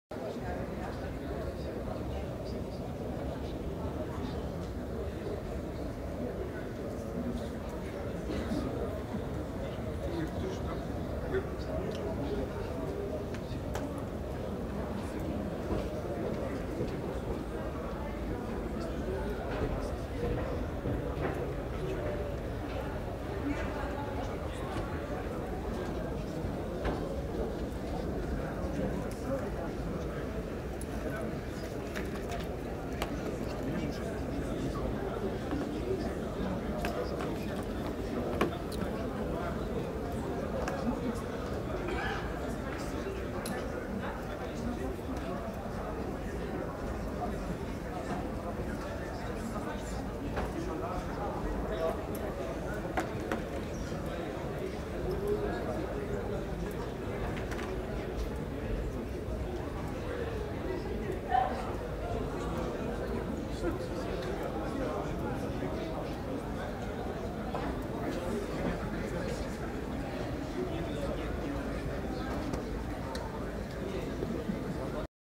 Общероссийский гражданский форум. Часть 3
Центр Международной Торговли, Конгресс-центр
В поисках новой модели социального государства